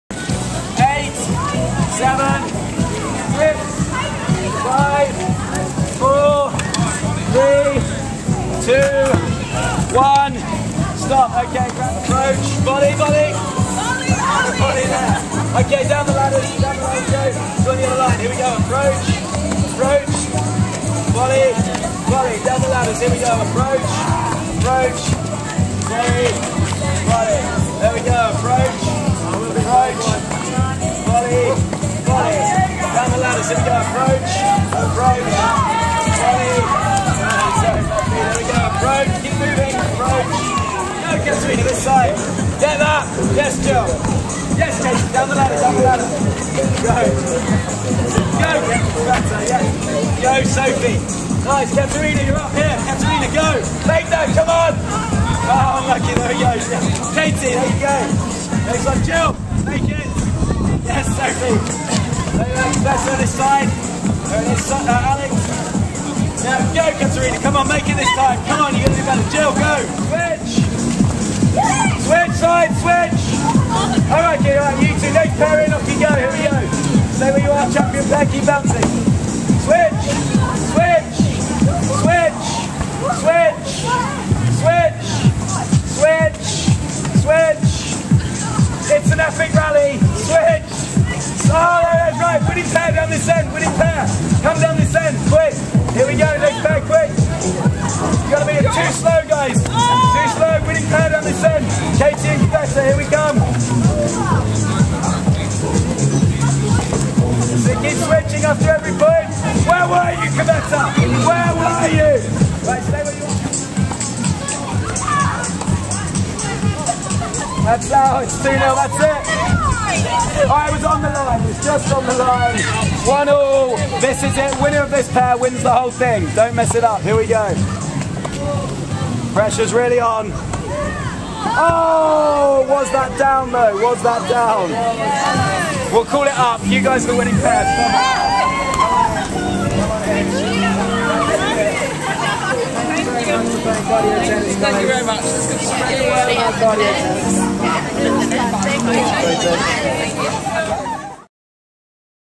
Kveta Peschke & Katarina Srebotnik play cardio tennis
Kveta Peschike & Katarina Srebotnik play cardio tennis at the AEGON International